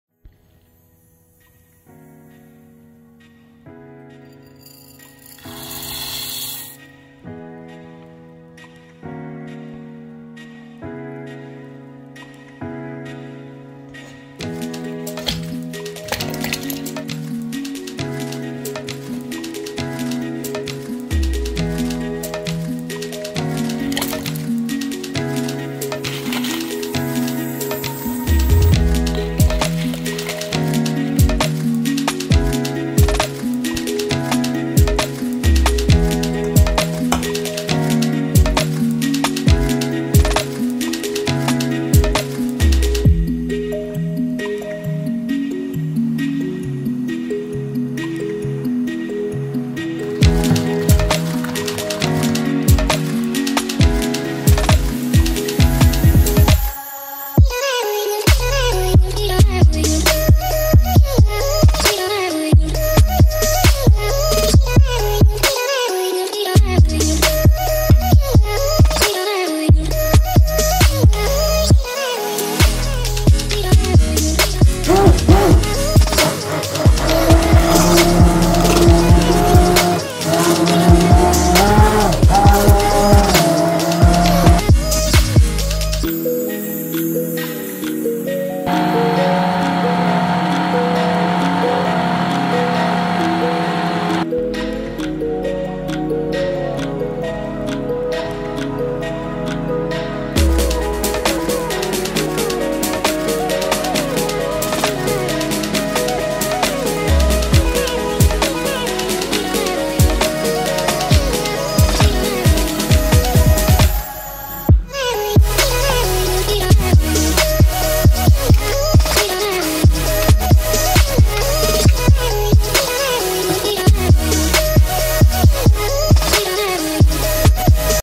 Reteta Supa crema de post cu legume
Passion for cooking iti arata cum se face supa crema de legume pas cu pas!
Aceasta reteta de supa crema de legume este ideala pentru cei ce prefera o dieta vegetariana de post și nu numai!
Supa crema de legume servita alaturi de crutoane aromate este perfecta intr-o zi de post sau cand vrei sa mananci ceva usor!

Ingrediente:
2 cartofi
2 morcovi
2 cepe
2 dovlecei
1 pastarnac
sare și piper

100g. linte roșie